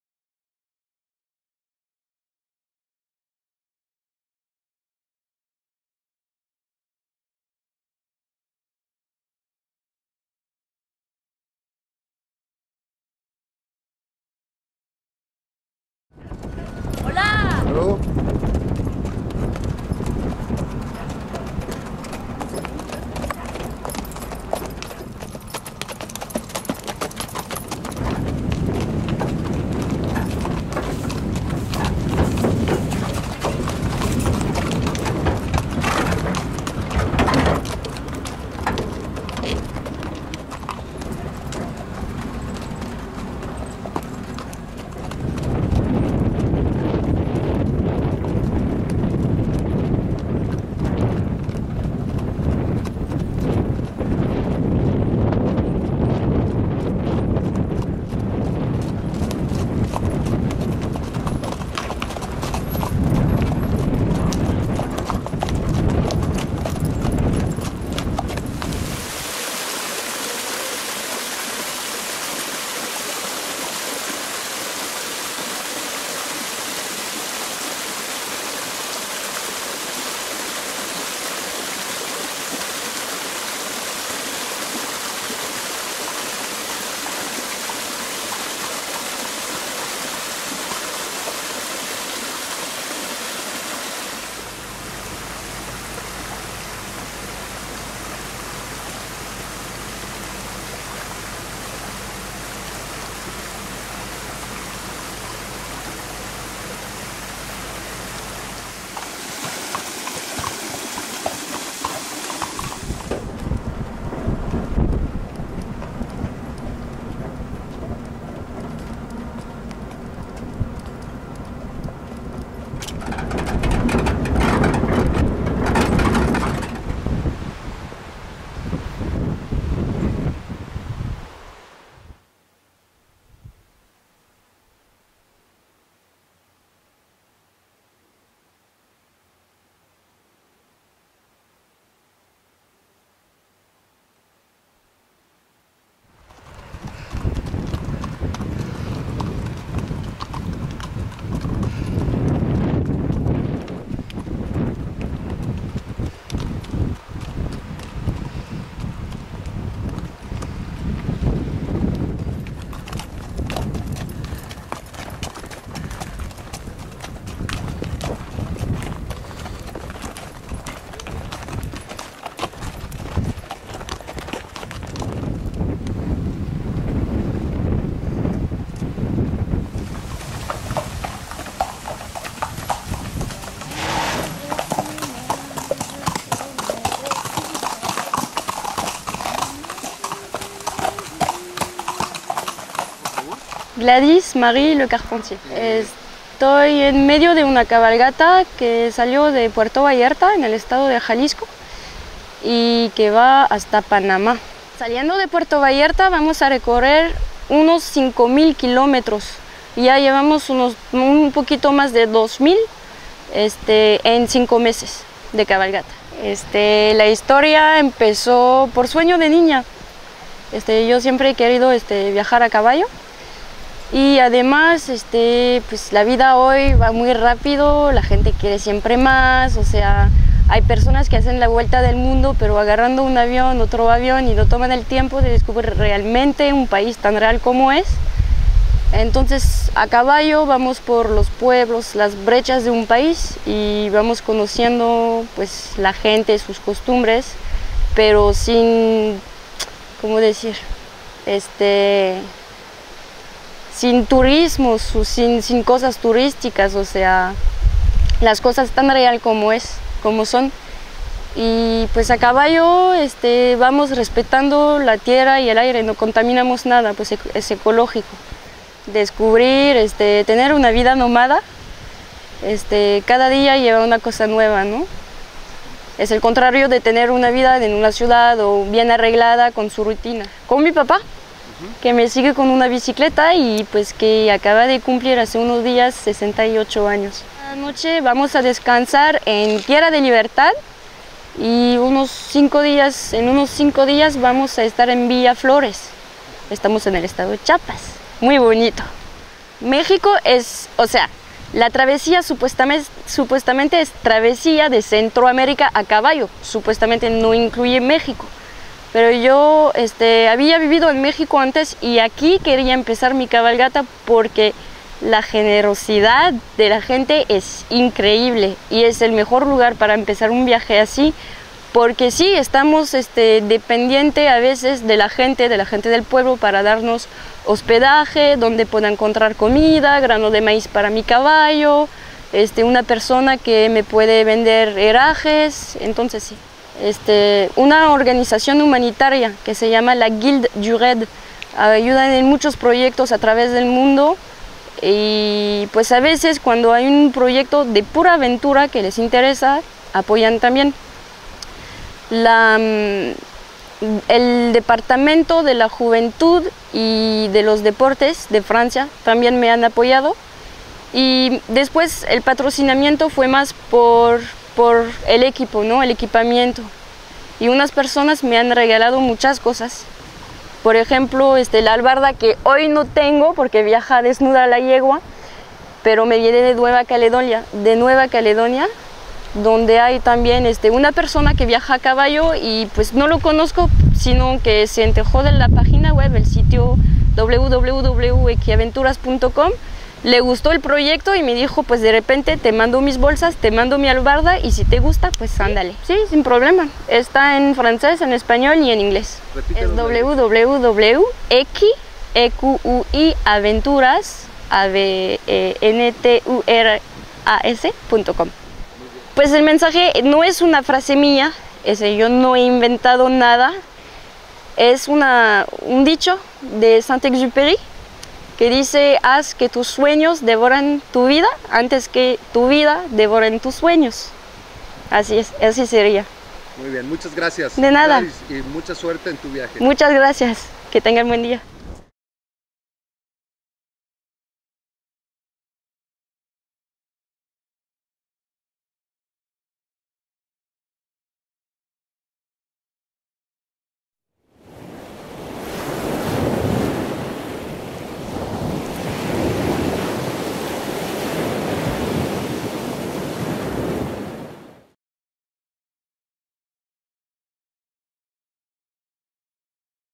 2 LA FRANCESA - Equiaventuras
Harta de una sociedad sin valores, una extraordinaria chica francesa, Gwladys Lecarpentier, decide cruzar México y Centro América a caballo en una aventura de las grandes. Una gran historia de amor y desafío que ennoblece al los humanos y a los caballos. Una maestra en el arte de vivir.

"... Dos fulgores que nunca se apagan,
caballo con duende y jovialidad de doncella..."

!!EN HORA BUENA!!